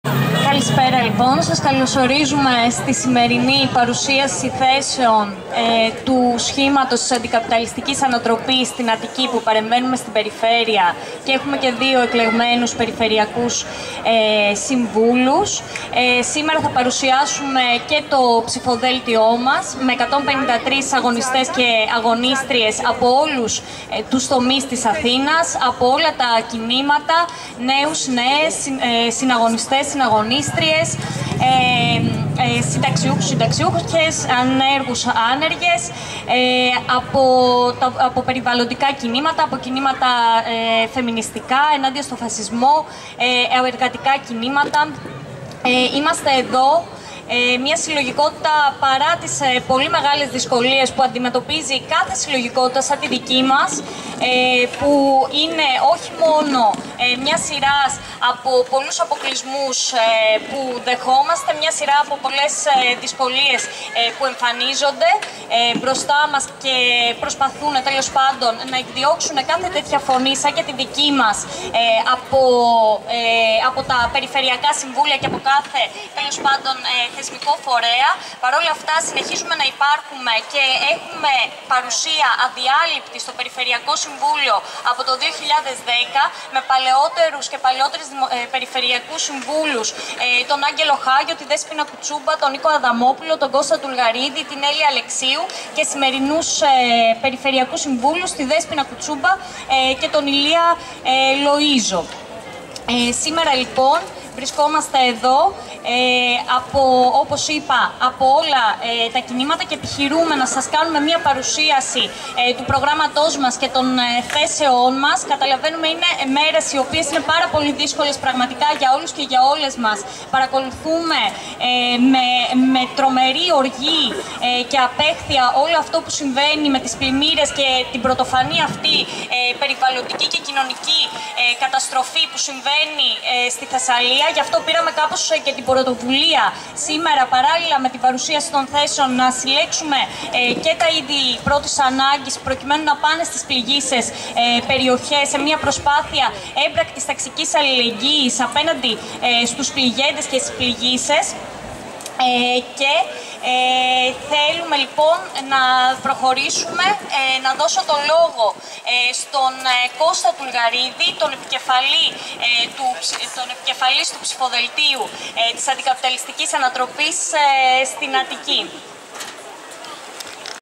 ΑΑΑ –Πλ. Αυδή, 10/9/23: Κωνσταντίνα Γκόντρα
Απόσπασμα εκδήλωσης που πραγματοποίησε η Αντικαπιταλιστική Ανατροπή στην Αττική, την Κυριακή 10 Σεπτέμβρη 2023 στην Αθήνα, στην Πλατεία Αυδή, για την παρουσίαση των Θέσεών της ενόψει των περιφερειακών εκλογών της 8ης Οκτώβρη. Στο συγκεκριμένο βίντεο παρακολουθούμε το άνοιγμα της εκδήλωσης από την συντονίστρια Κωνσταντίνα Γκόντρα.